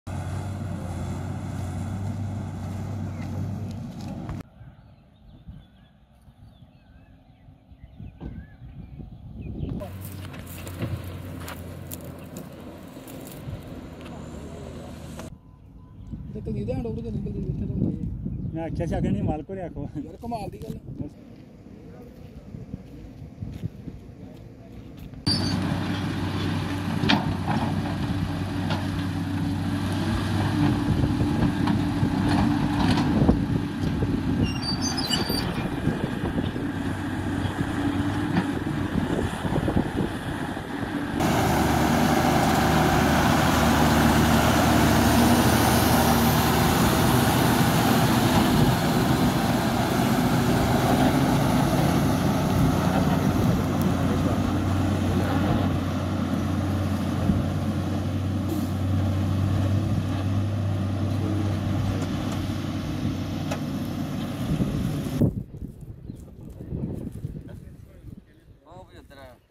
Lakhanpur Police Seized Poclain Machine and a dumper doing Illegal Mining in Maggar Khad area
Kathua | Lakhanpur police seized Poclain Machine and a dumper doing illegal mining in Maggar Khad adjacent to Maroli village